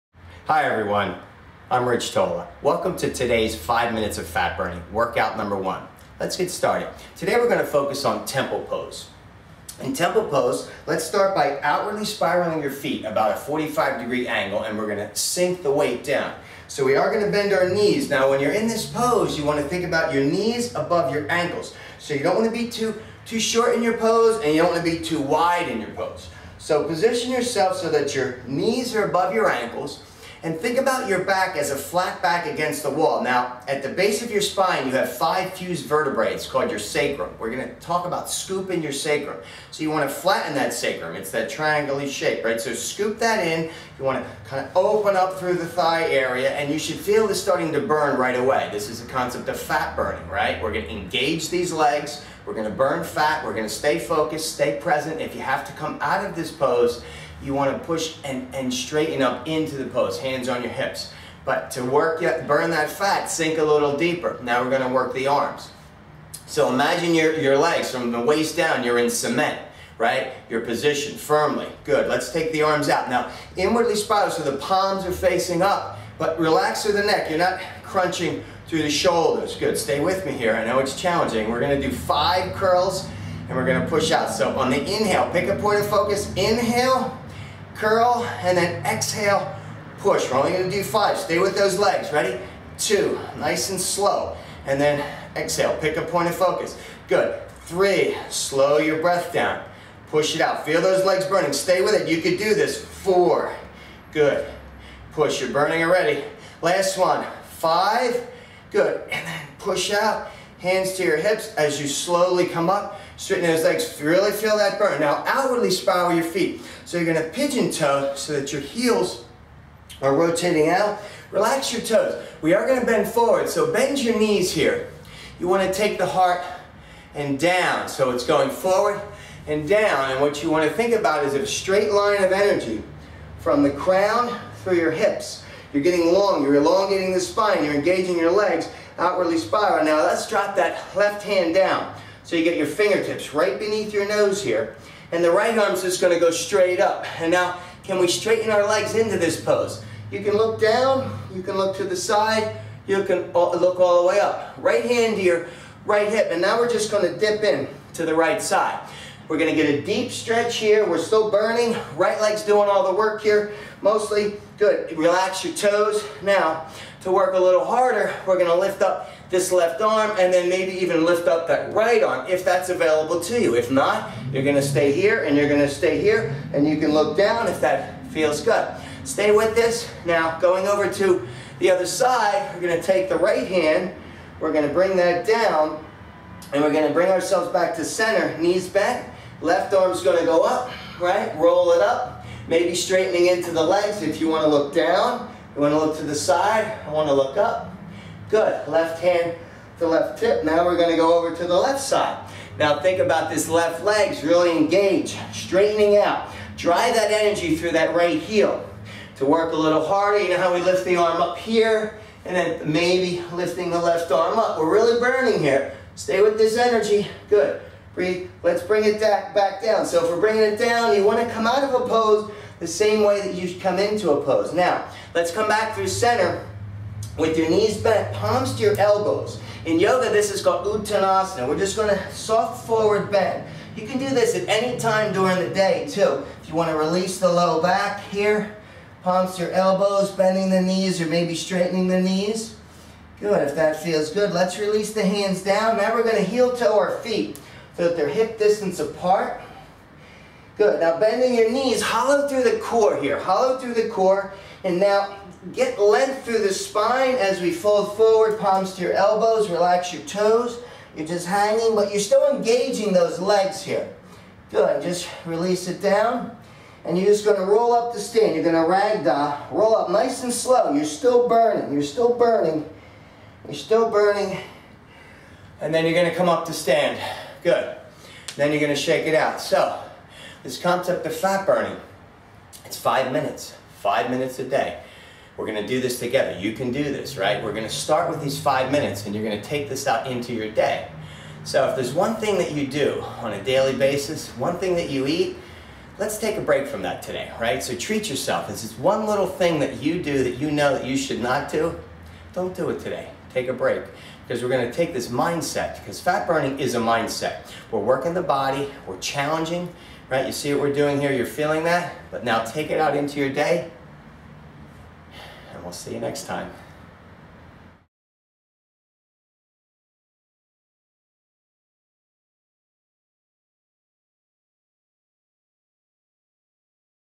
Beautify Your Thighs - Rich Tola's 5 Minute Fat Burning Workout #1
Here's the first episode of my 5 Minute Fat Burning Workouts (2011): 100 Workouts in 100 Days. Every workout is safe and easy-to-follow and uses poses and stretches based on Yoga

All Tola Yoga classes, my fitness DVDs, film, memoir, talk show, podcast and more All social: @richtola